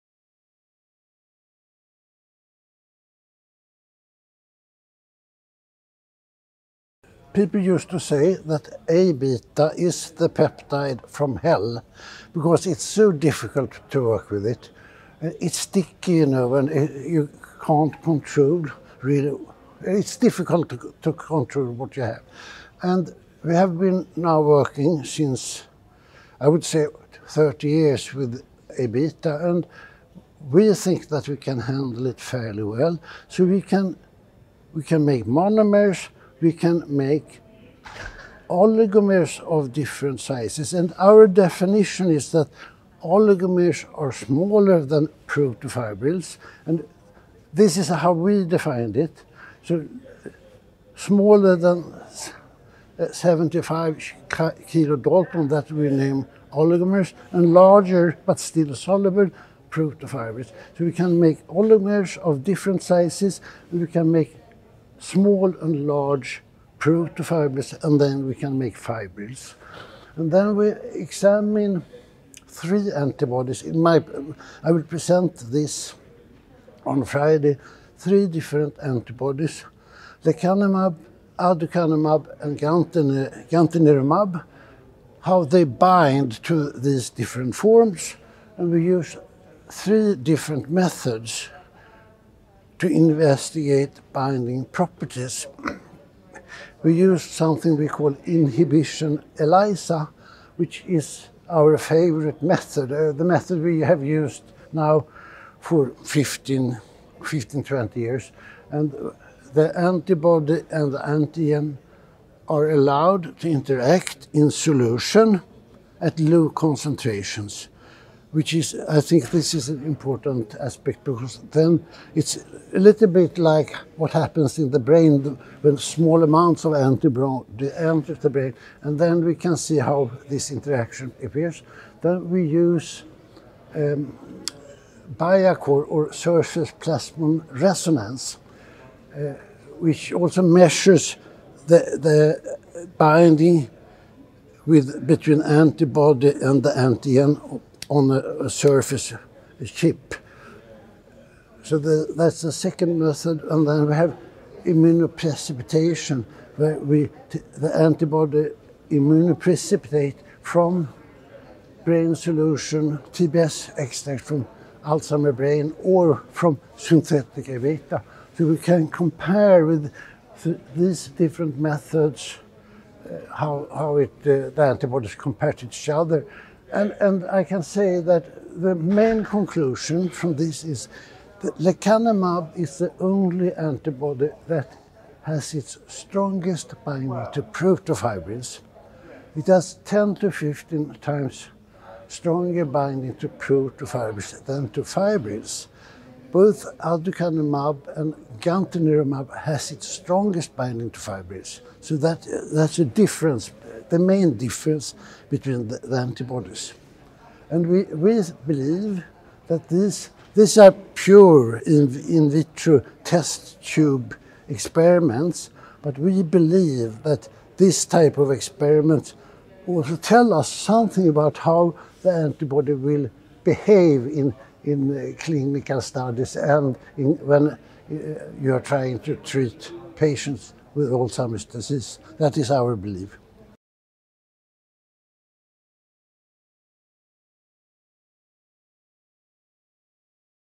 Comparing binding characteristics to Aβ of lecanemab, aducanumab & gantenerumab
A spectrum of aggregated amyloid β (Aβ) species, ranging from dimers, oligomers, protofibrils, and insoluble fibrillar Aβ, exist in a complex equilibrium in the Alzheimer's disease brain. The most toxic forms of Aβ are the lower molecular weight species known as Aβ oligomers and protofibrils. Through decades of research in an attempt to develop treatment strategies, immunotherapy against Aβ has emerged as a promising treatment for Alzheimer's disease. Lars Lannfelt, MD, PhD, Uppsala University, Uppsala, Sweden, discusses a study comparing the binding characteristics to Aβ of three antibodies, lecanemab, aducanumab, and gantenerumab, by three different methods: inhibition ELISA, immunodepletion and Surface Plasmon Resonance. The main difference observed between the antibodies was that lecanemab has its strongest binding to protofibrils, one of the most toxic forms of Aβ, while aducanumab and gantenerumab bind strongly to fibrils. Prof. Lannfelt highlights that in vitro studies such as the one discussed are essential to infer how the antibodies behave in clinical studies. This interview took place at the AD/PD™ 2022 Conference in Barcelona, Spain.